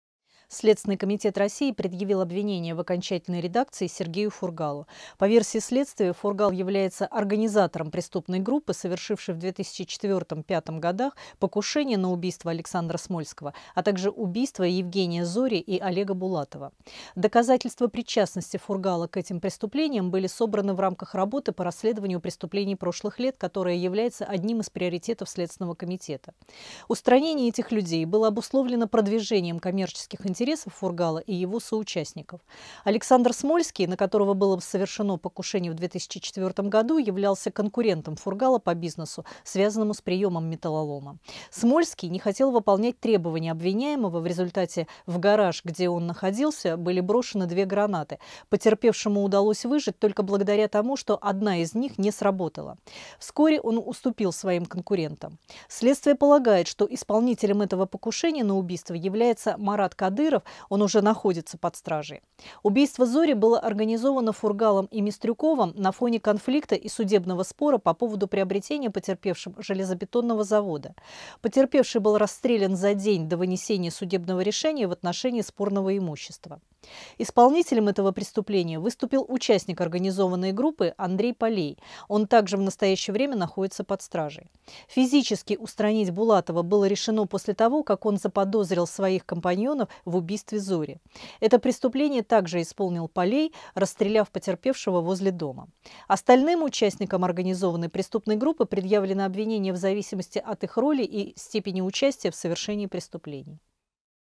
Фургалу предъявили окончательное обвинение
Следственный комитет России предъявил бывшему губернатору Хабаровского края Сергею Фургалу обвинение в окончательной редакции. Чиновника обвинили в убийствах и хранении оружия, передает официальный представитель СК РФ Светлана Петренко.
«…Покушение на убийство двух и более лиц, совершенное организованной группой, из корыстных побуждений, по найму, общеопасным способом, убийство двух и более лиц, совершенное организованной группой, по найму, с целью скрыть другое преступление, незаконное приобретение, хранение, ношение огнестрельного оружия, боеприпасов и взрывных устройств, совершенное организованной группой», — говорится в заявлении ведомства.
Следователи полагают, что Сергей Фургал основал преступную группу, которая в 2004—2005-х годах попыталась убить предпринимателя Александра Смольского, а также бизнесменов Евгения Зори и Олега Булатова. Таким образом экс-глава Хабаровского края пытался расширить свой бизнес — чтобы открыть новые пункты приема металлолома в Амурской области, ему требовалось устранить конкурентов.
Напомним, Сергея Фургала задержали 9 июля прошлого года. После этого Хабаровск захлестнула волна протестов.